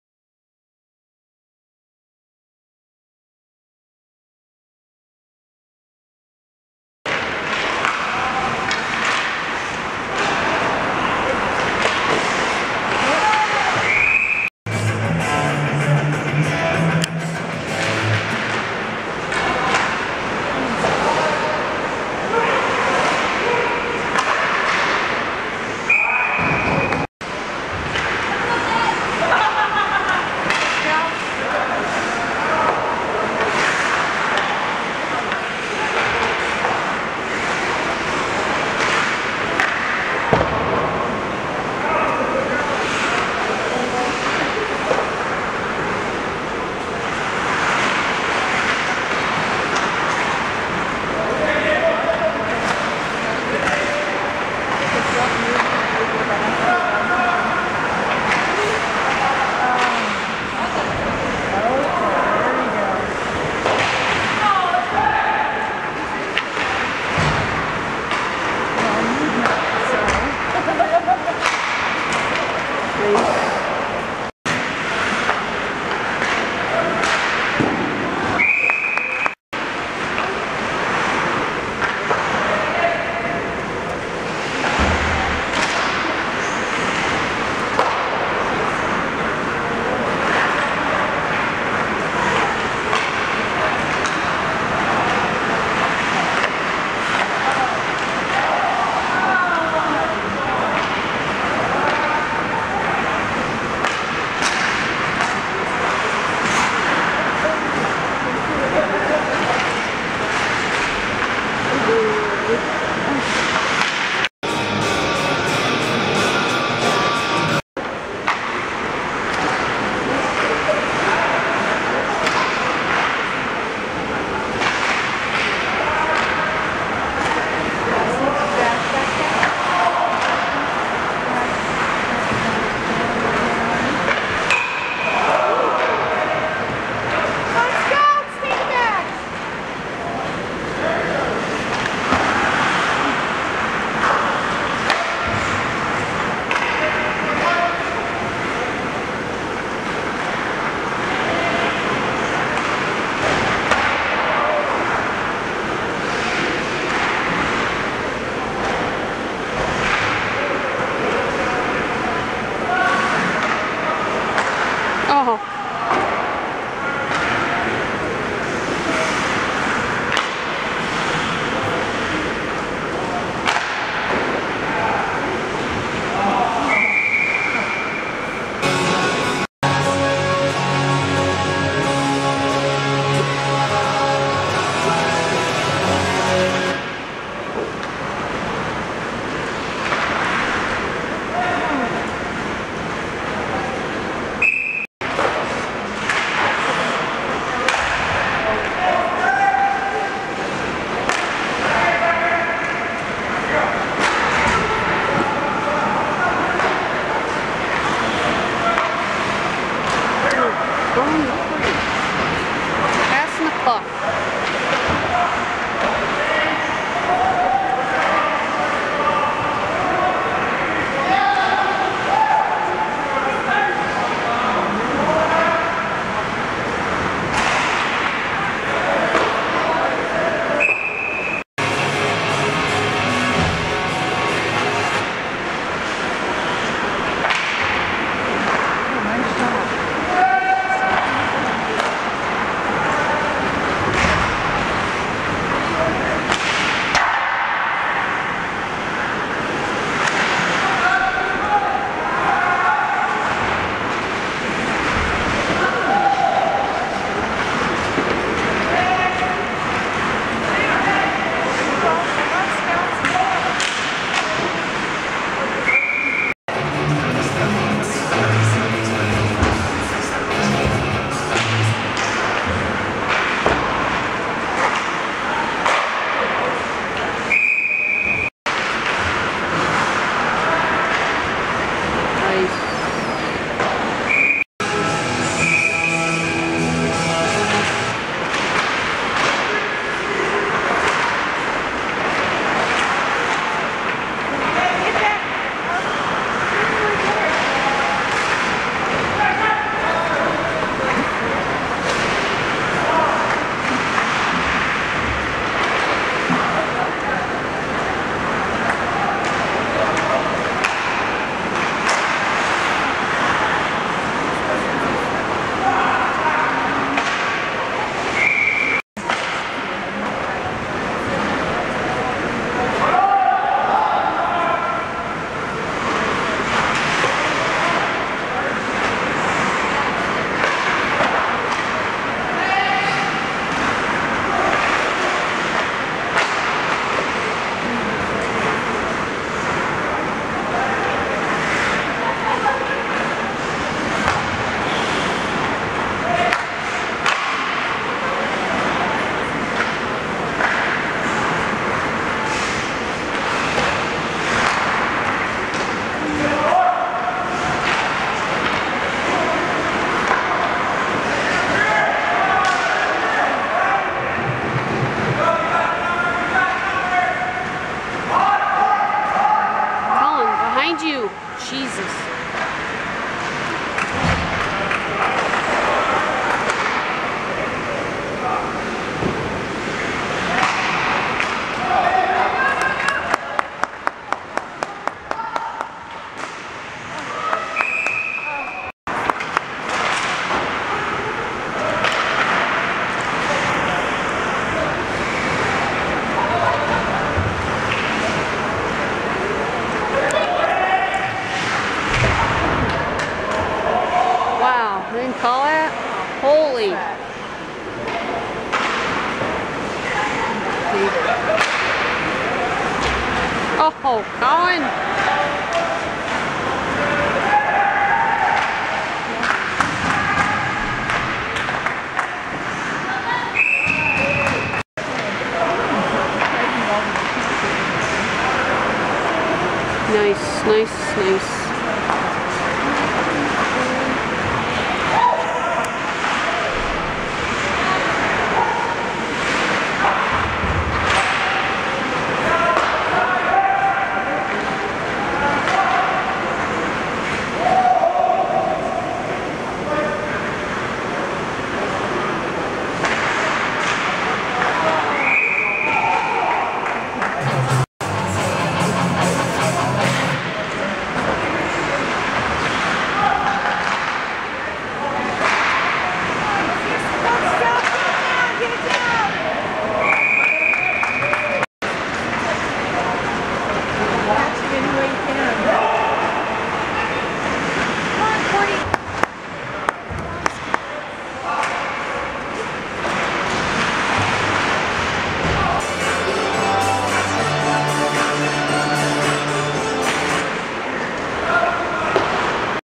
ColinHockey30Oct2012_2
Colin Miller playing hockey at the Westminster Promenade.  Westminster, Colorado.  October 30, 2012.  The Scouts